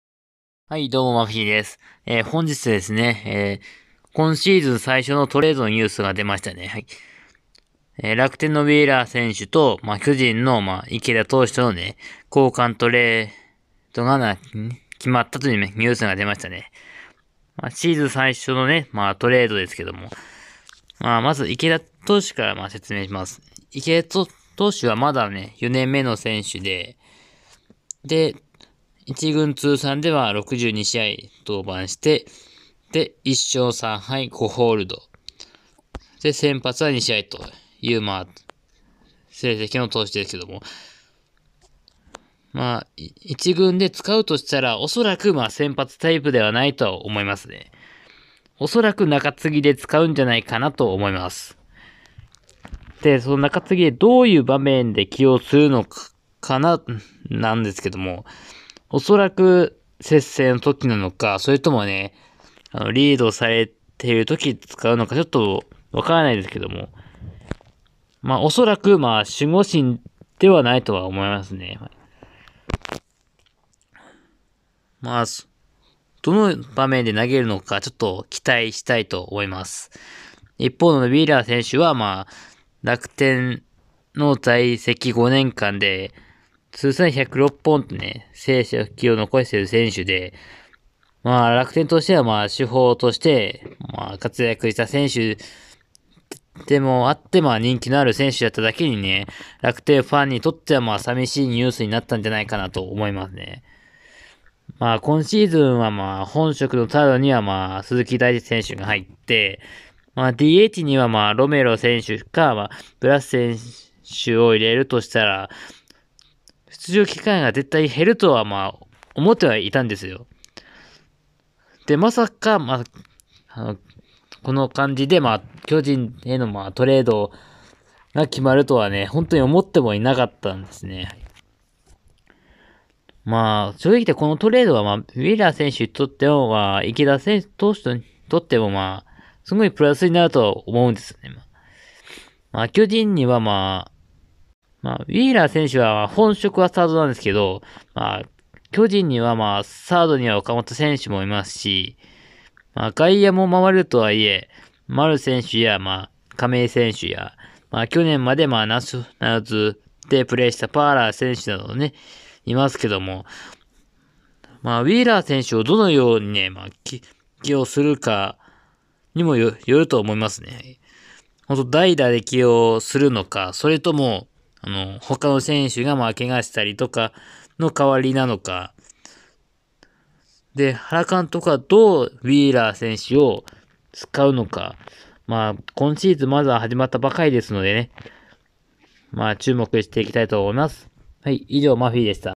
どう使う？楽天ウィーラー選手と巨人池田投手のトレード
２０２０年初のトレード情報
楽天ウィーラー選手　巨人　池田駿投手
交換トレード成立のニュース

両選手をどう起用するのか？

この動画を気に入ってくださったら